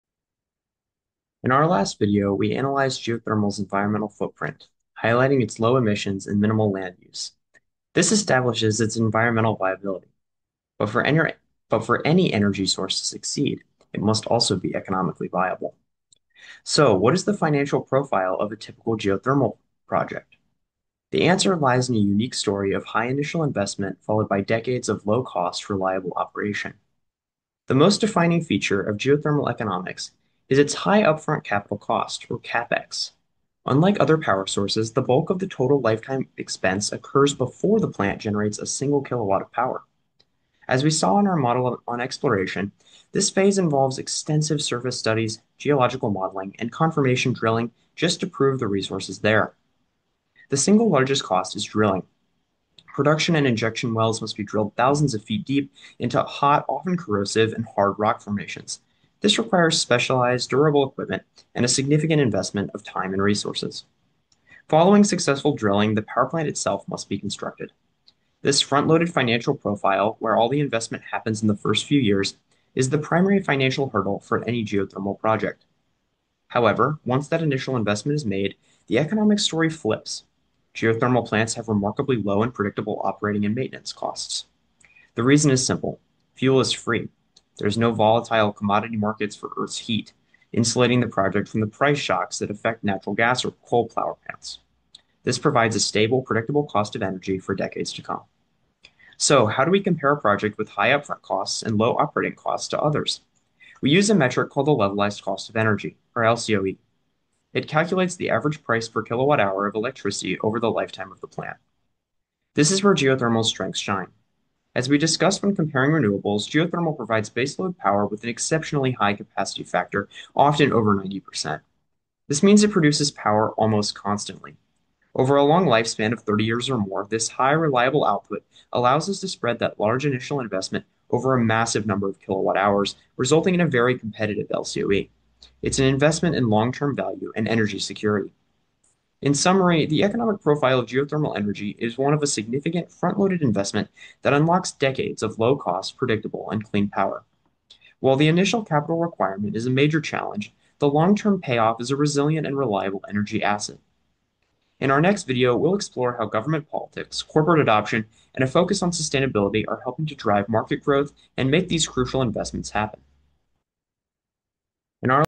Economics of Geothermal
Is geothermal energy affordable? We know it's a clean and reliable powerhouse, but what's the real cost? In this video, we dive into the complete financial profile of a typical geothermal project.

We break down the unique economic story of geothermal power, starting with the high upfront capital costs (CAPEX) required for exploration, deep drilling, and plant construction. Then, we explore the long-term payoff: the remarkably low and stable operating costs that come from using a free fuel source—the Earth's heat.

Finally, we tie it all together with the Levelized Cost of Energy (LCOE), the key metric for comparing energy sources. Learn how geothermal's high capacity factor and long operational lifespan make it a competitive and secure energy investment for the future.

This video is part of a comprehensive course on geothermal energy. Check out the full series: